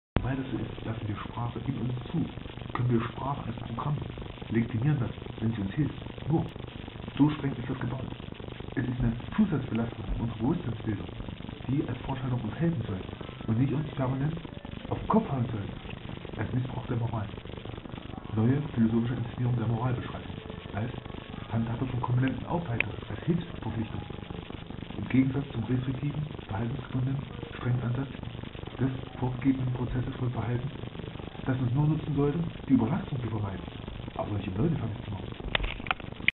Die Sprache ist ein gutes Hilfsprogramm!
Die Sprache ist nur ein Hilfsprogramm der Bewusstseinsausführung!
Die Sprache ist ein von uns entworfenes zusätzliches Hilfprogramm zur Lebensausführungsentwicklung, nicht mehr aber auch nicht weniger. Philosophen überprüfen nicht nur die Strengzeichnungsgrundlagen der logischen Kalküle, bauen nicht nur die Wertentwicklungsmoeglichkeiten und Update-Funktionsgarantien, sondern Überprüfen auch die restriktiven Ausführungsarten der Sprache als dominante ( sie wird sich mittlerweile wohl allein vorkommen fühlend - dort im solipsistischen Bewusstseinsansatz alles andere verdrängen könnenedes Anweisungsbefehlrumbellereiprozedere - Ha, Ha, Ha ) Bewusstseinskomponente.